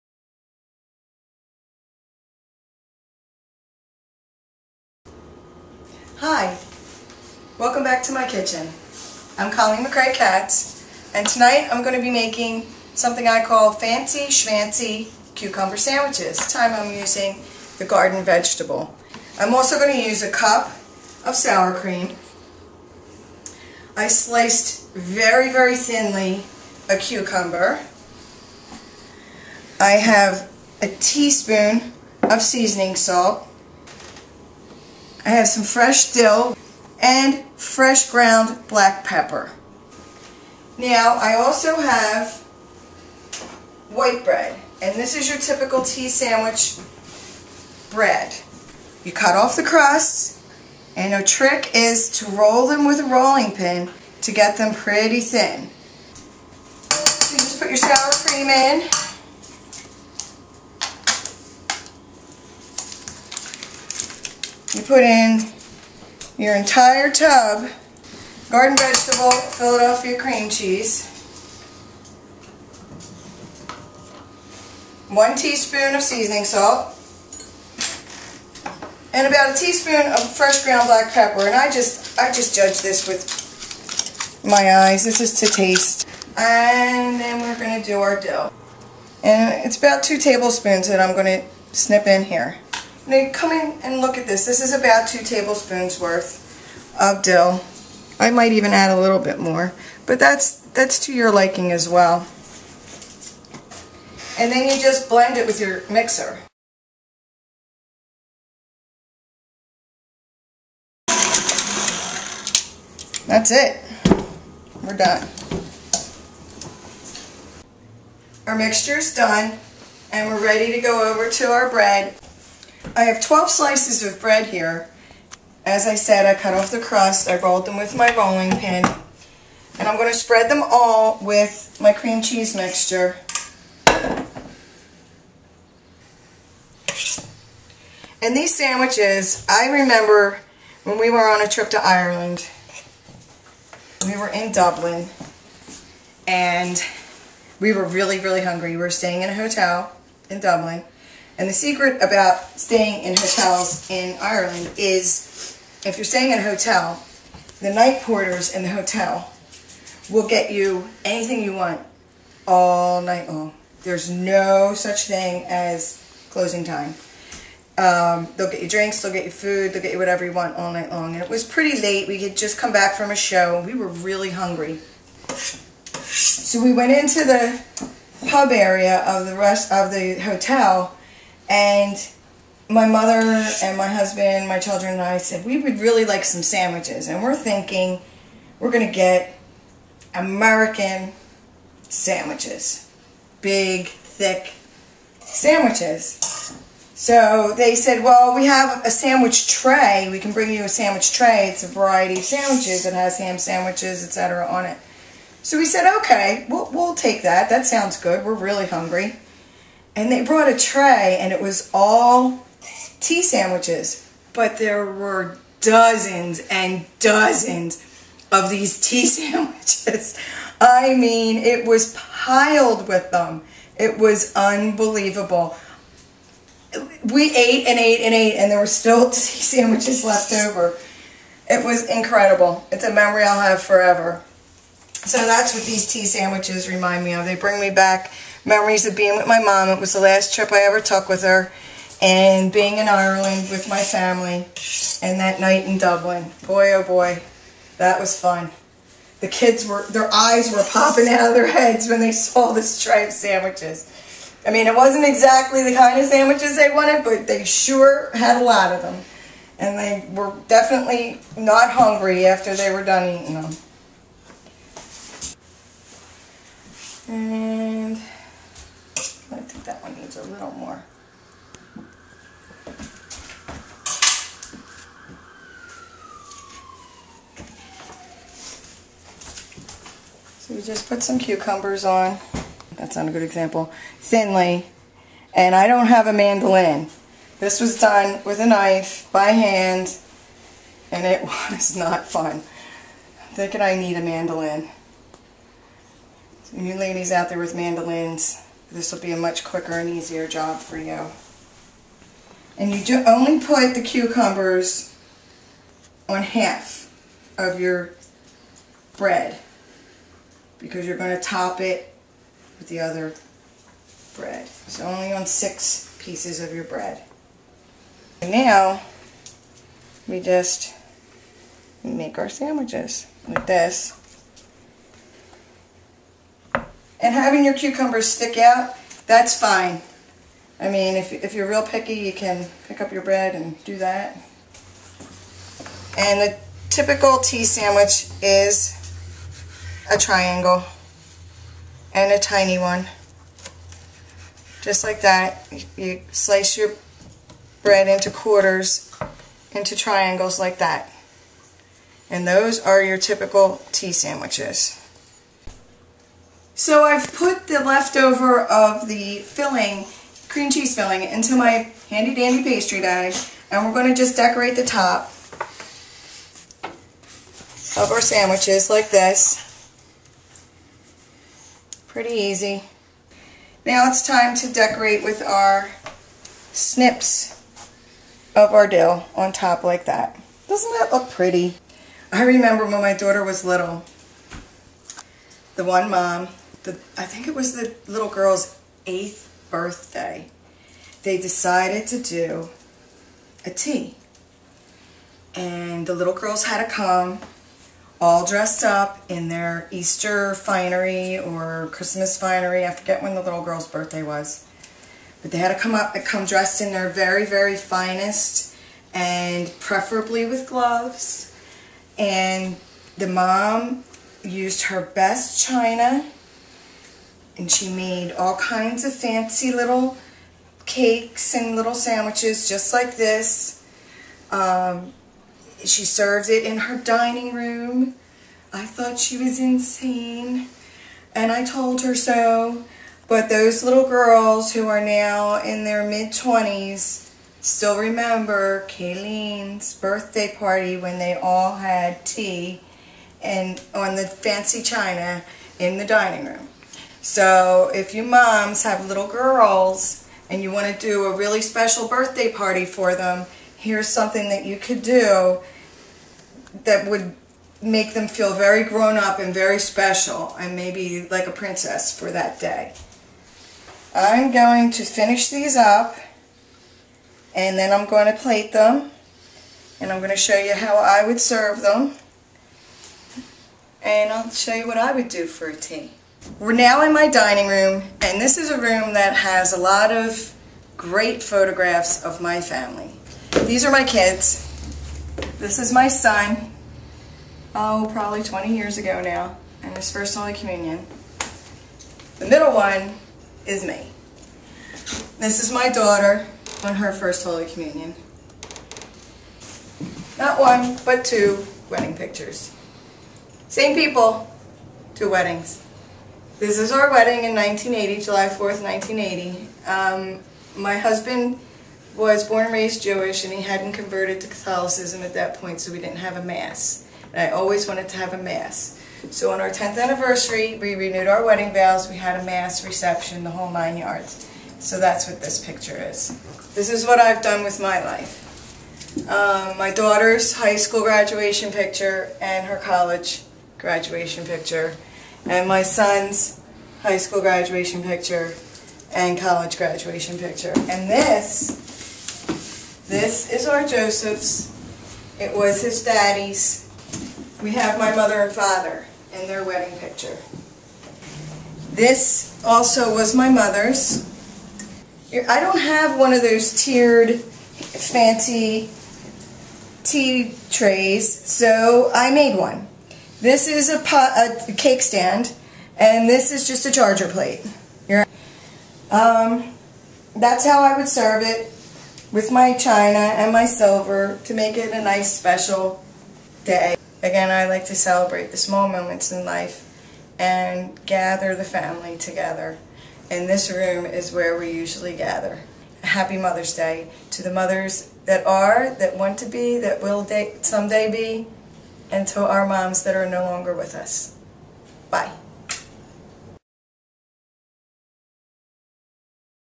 Cucumber Sandwiches.wmv
These are very fancy little tea sandwiches that you can serve for tea, as appetizers, for brunch, whatever!  These would be perfect for Mother's Day or to make your favorite little girl feel very grown up!!!